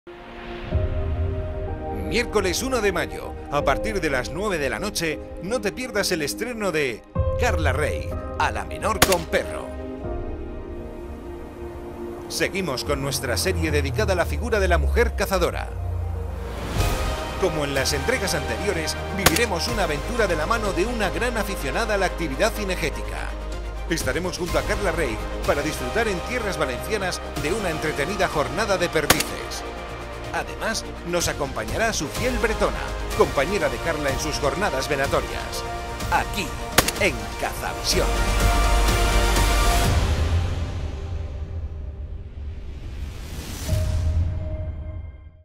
Carla Reig, a la menor con perro. Miércoles de 1 Mayo a las 21:00.
Seguimos con nuestra serie dedicada a la figura de la mujer cazadora. Como en las entregas anteriores, viviremos una aventura de la mano de una gran aficionada a la actividad cinegética. Estaremos junto a Carla Reig para disfrutar en tierras valencianas de una entretenida jornada de perdices. Además, nos acompañará su fiel bretona, compañera de Carla en sus jornadas venatorias.
¡el primer mes es gratis!